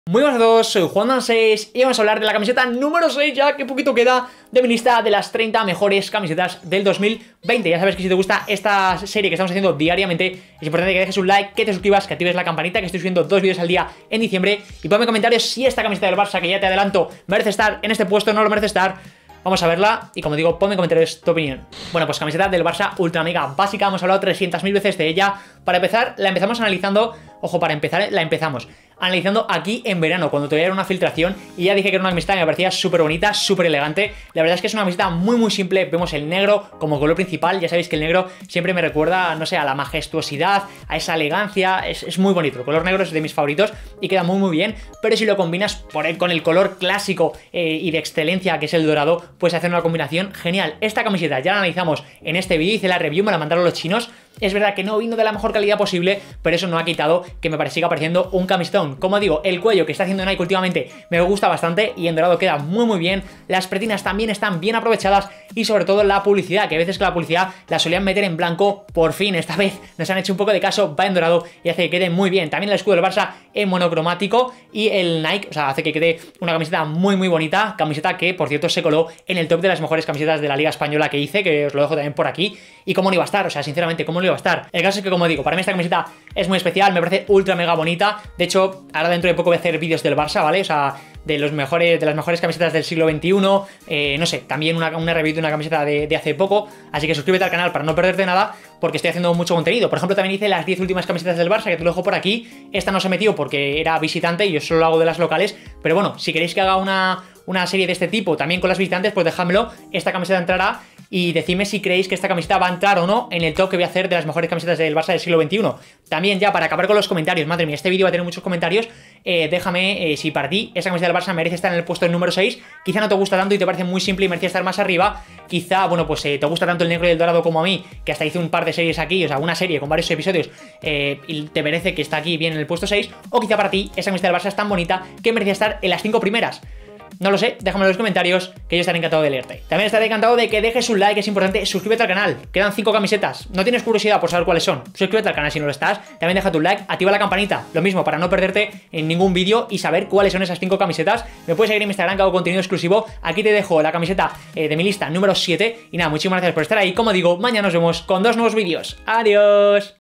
#6 Las 30 MEJORES CAMISETAS de FÚTBOL *del 2020*
Continuamos la serie de las 30 CAMISETAS de FÚTBOL más bonitas de este año (2020)